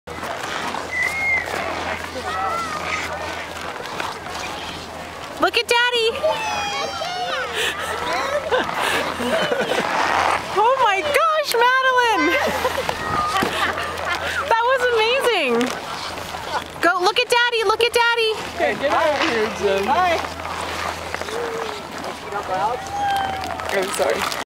Oliver Skiing
Oliver's first day skiing at Keystone Dec. 27, 2010.